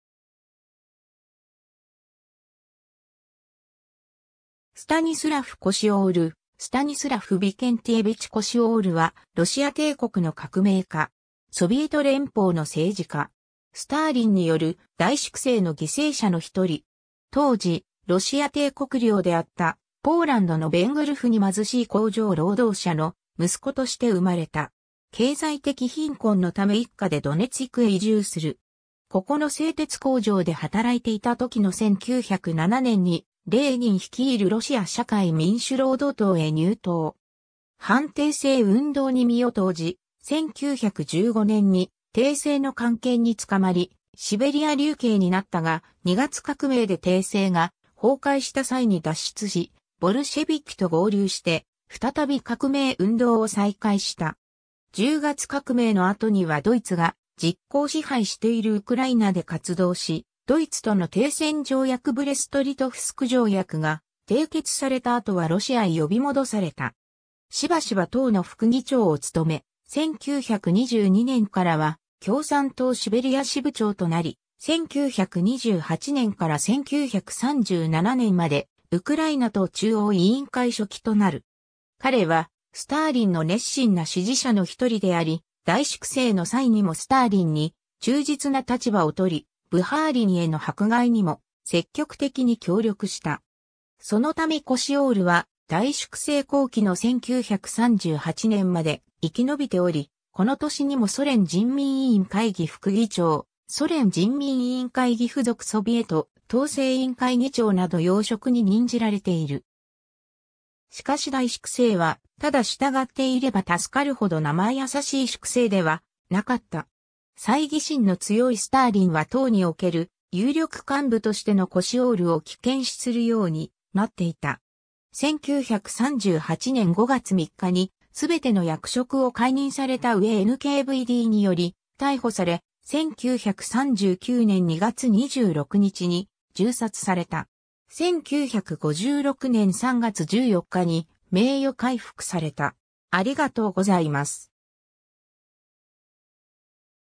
スタニスラフ・コシオール
全連邦共産党（ボリシェヴィキ）中央委員会組織局の人物
スタニスラフ・コシオール スタニスラフ・ヴィケンティエヴィチ・コシオール（ロシア語: Станислав Викентьевич Косиор 英語: Stanislav Vikent`evich Kosior、1889年11月18日‐1939年2月26日）は、 ロシア帝国の革命家。
ソビエト連邦の政治家。
スターリンによる大粛清の犠牲者の一人。
 当時、ロシア帝国領であったポーランドのヴェングルフ（Węgrów）に貧しい工場労働者の息子として生まれた。
経済的貧困のため一家でドネツィクへ移住する。
ここの製鉄工場で働いていた時の1907年にレーニン率いるロシア社会民主労働党へ入党。
反帝政運動に身を投じ、1915年に帝政の官憲に捕まり、シベリア流刑になったが、2月革命で帝政が崩壊した際に脱出し、ボルシェヴィキと合流して再び革命運動を再開した。
10月革命の後にはドイツが実効支配しているウクライナで活動し、ドイツとの停戦条約ブレスト＝リトフスク条約が締結されたあとはロシアへ呼び戻された。
しばしば党の副議長を務め、1922年からは共産党シベリア支部長となり、1928年から1937年までウクライナ党中央委員会書記となる。
 彼はスターリンの熱心な支持者の一人であり、大粛清の際にもスターリンに忠実な立場を取り、ブハーリンへの迫害にも積極的に協力した。
そのためコシオールは大粛清後期の1938年まで生き延びており、この年にもソ連人民委員会議副議長・ソ連人民委員会議付属ソヴィエト統制委員会議長など要職に任じられている。
しかし大粛清はただ従っていれば助かるほど生易しい粛清ではなかった。
猜疑心の強いスターリンは党における有力幹部としてのコシオールを危険視するようになっていた。
1938年5月3日にすべての役職を解任された上NKVDにより逮捕され、1939年2月26日に銃殺された。
 1956年3月14日に名誉回復された。